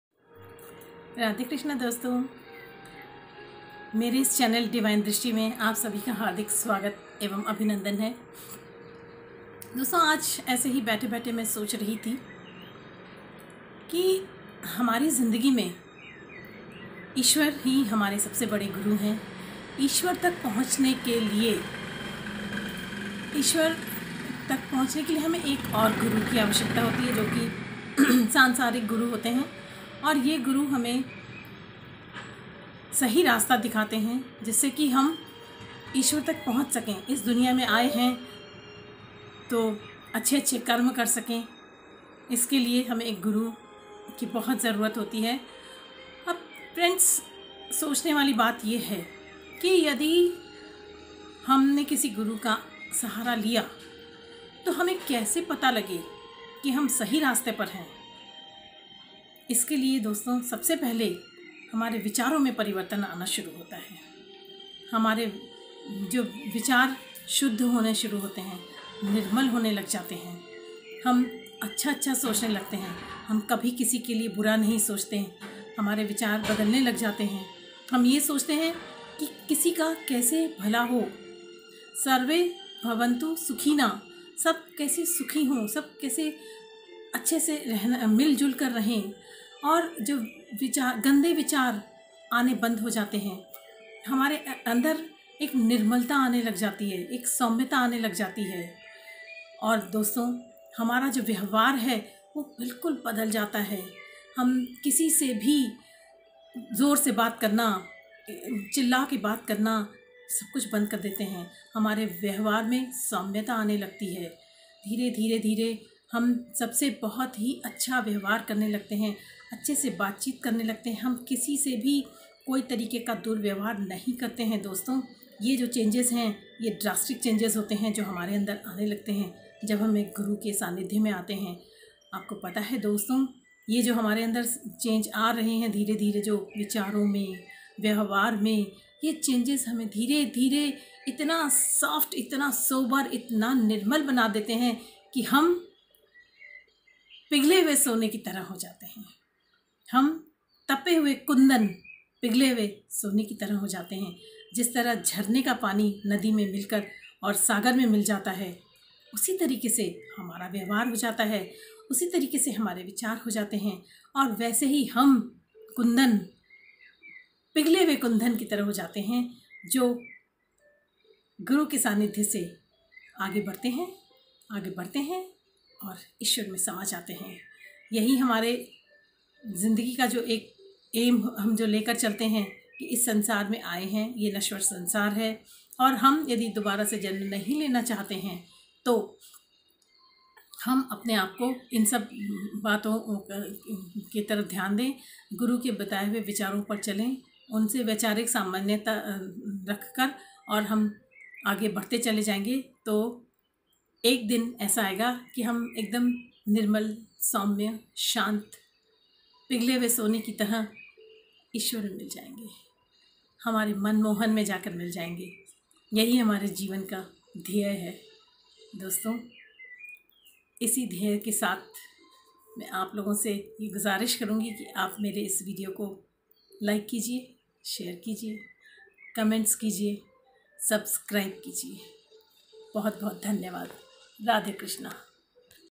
Guru ke saath ka prabhav
suniye, Guru ke prabhav se kaise hamare thoughts and behavior me changes aate hain.